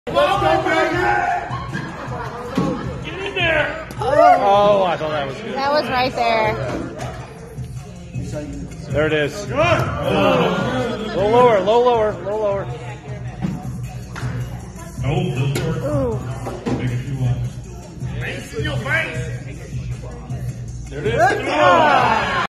Airmail Challenge #18
Cleveland Cornhole has an airmail pot that grows each week. Making one bag in the hole can make you anywhere from $50 to $3,000 a bag!